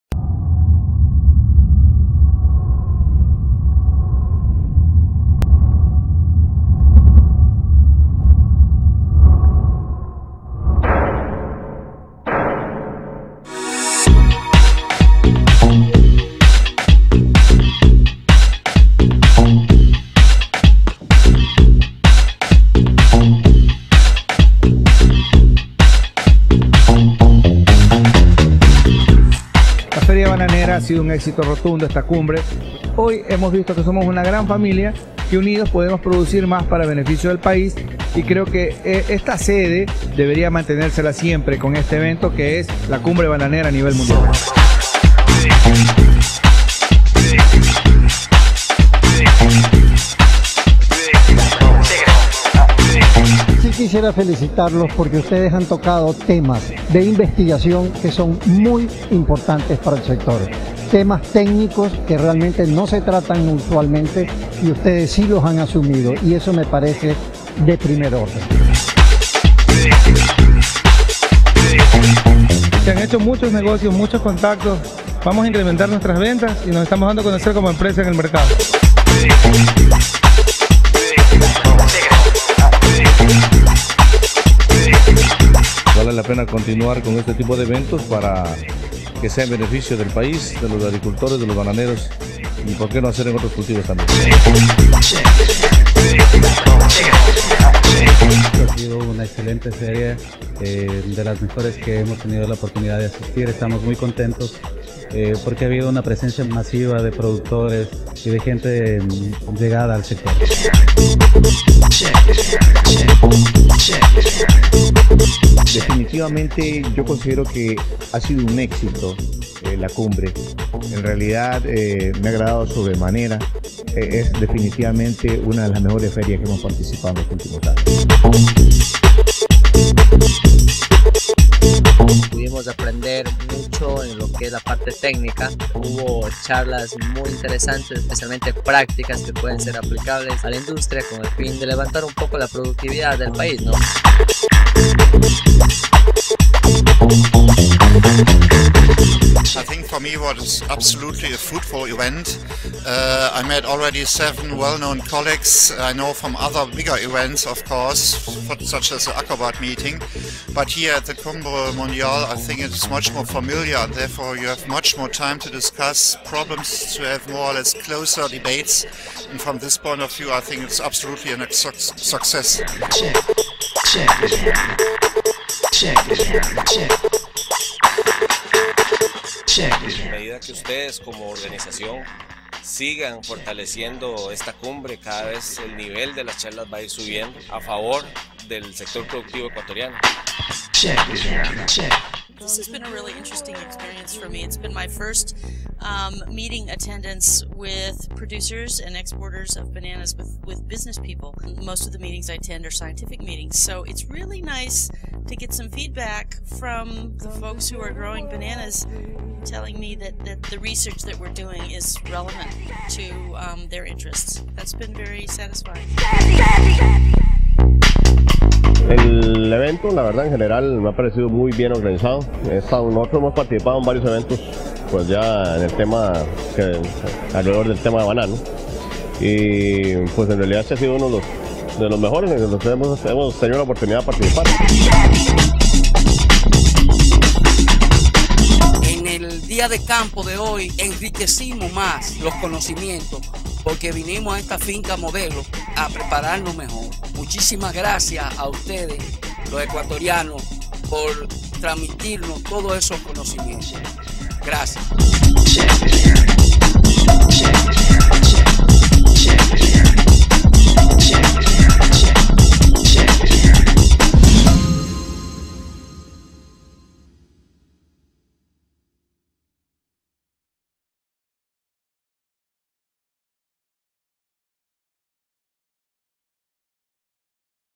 CUMBRE MUNDIAL DE BANANO 2011
El evento técnico, científico y comercial, más importante del sector; otra vez en Ecuador.